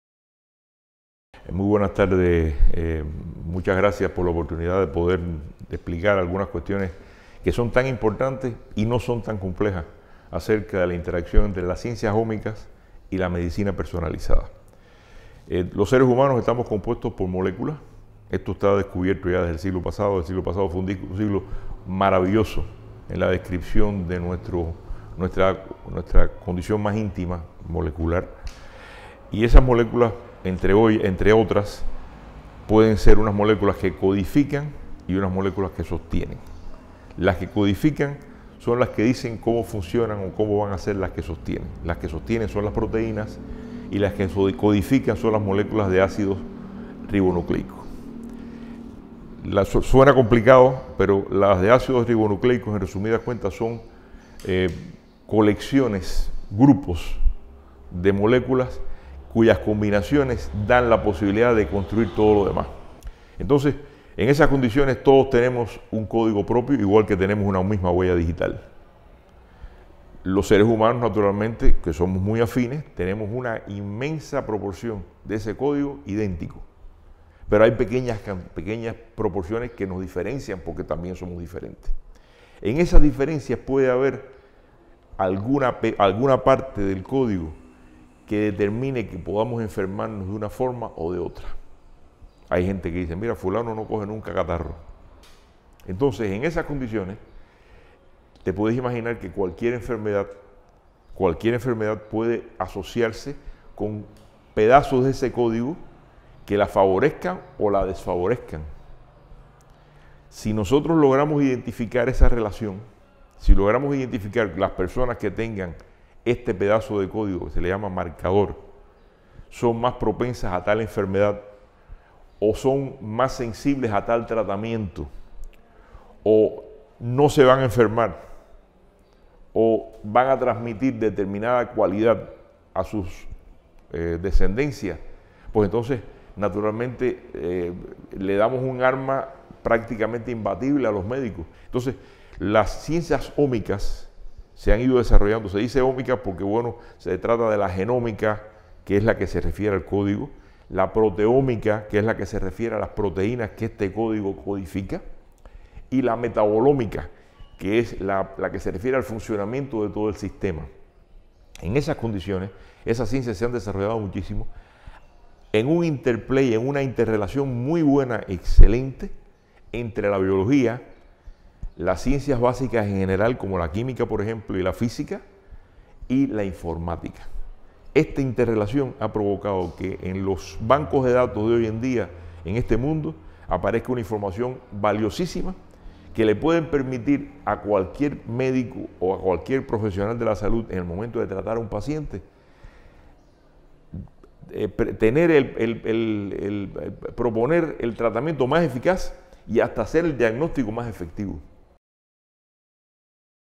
¿Qué son las ciencias ómicas en la medicina de precisión?
El Dr. Luis Alberto Montero nos comenta en las siguientes declaraciones sobre las ciencias ómicas, esenciales en la medicina de precisión.